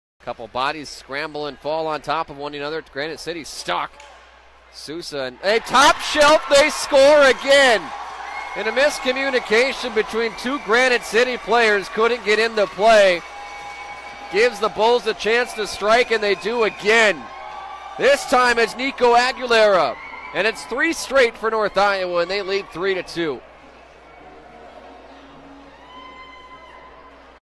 Highlight - Nico Aguilera Game Winner 3-7-2020
What a snipe from Nico Aguilera on the sharp angle Saturday night! This top-corner tally proved to be the game-winner in a 5-2 contest!